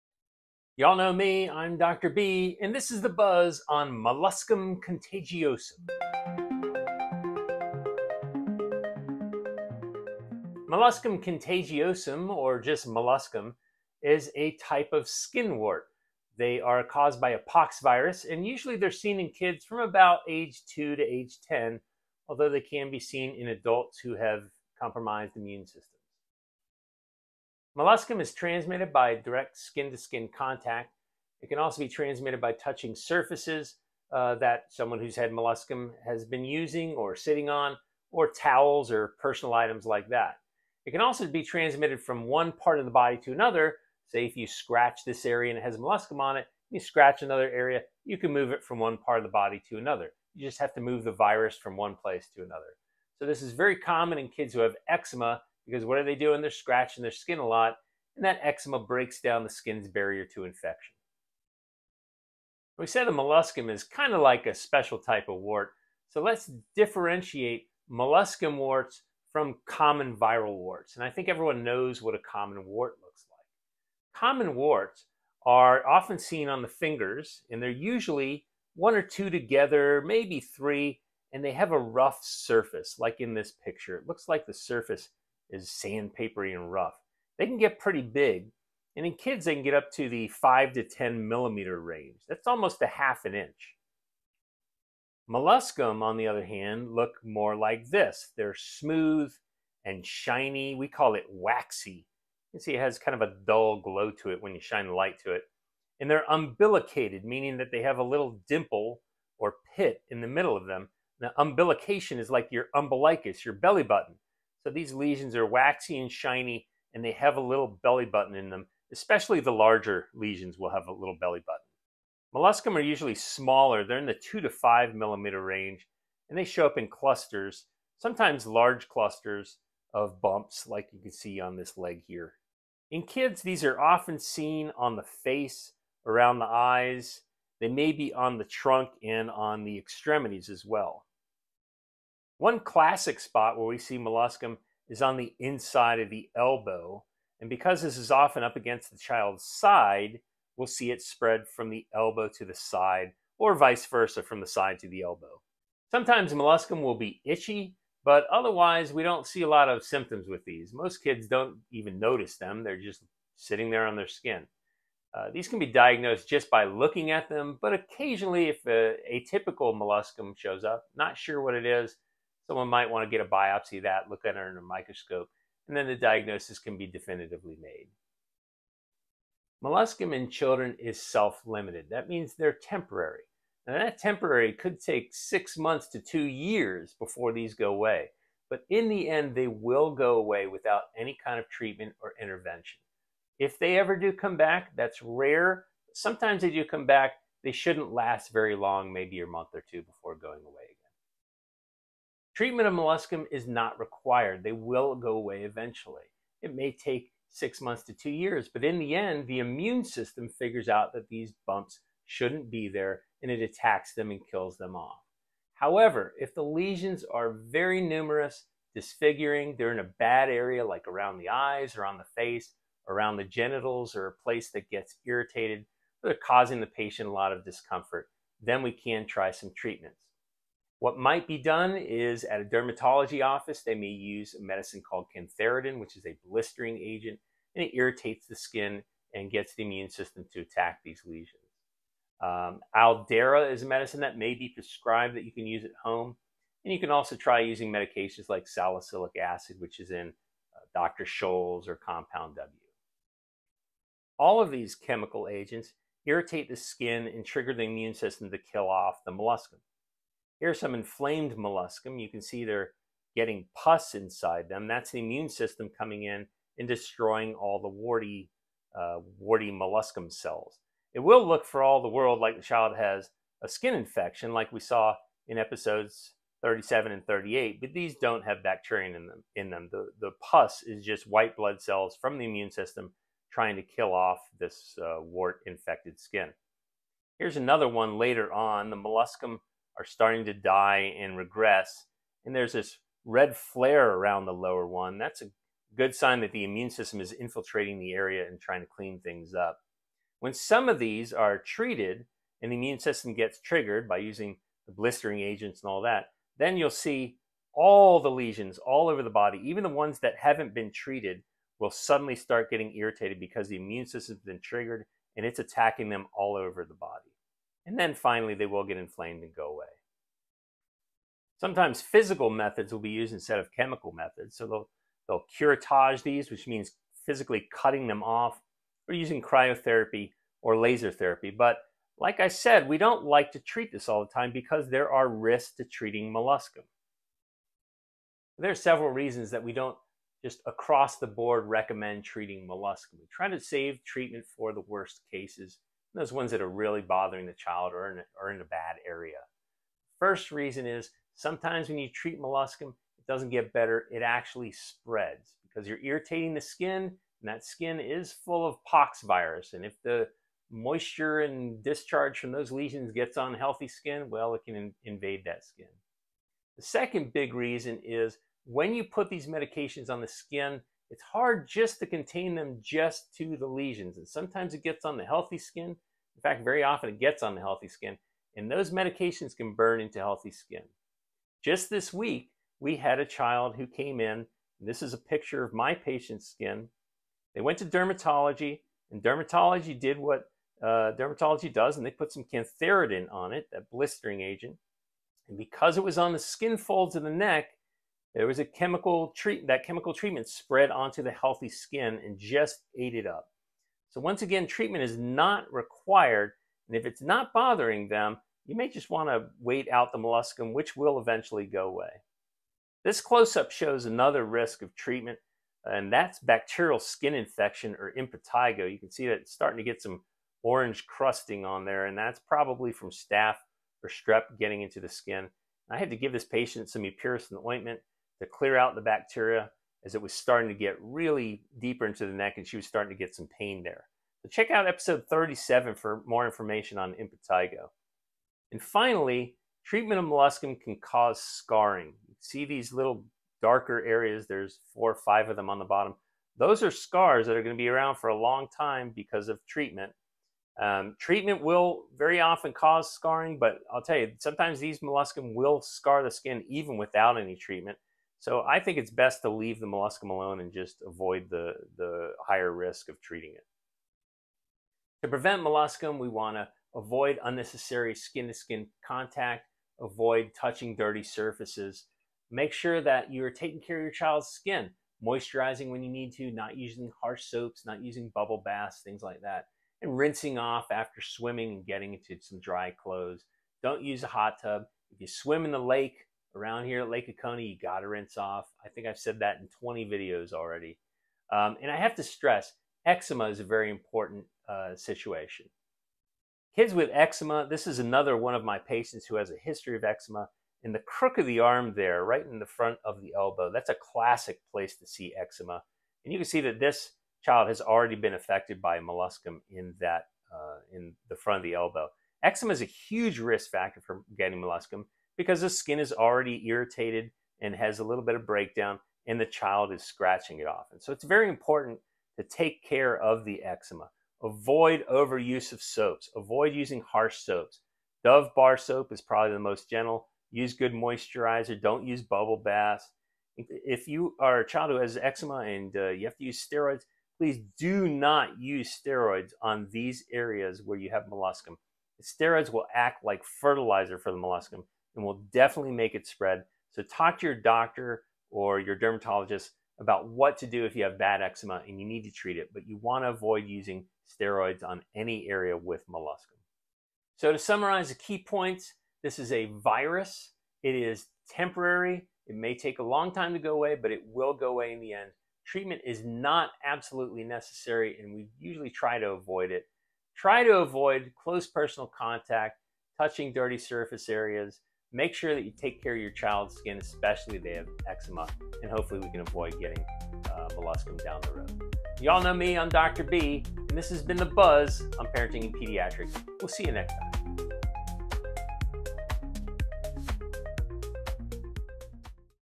Ep 42: Molluscum Contagiosum
Molluscum is a very common type of skin wart seen in children, that can last anywhere up to 2 years.  Learn how to recongnize the differences between molluscum and other types of warts, how we manage them and why treatment is not always the best idea!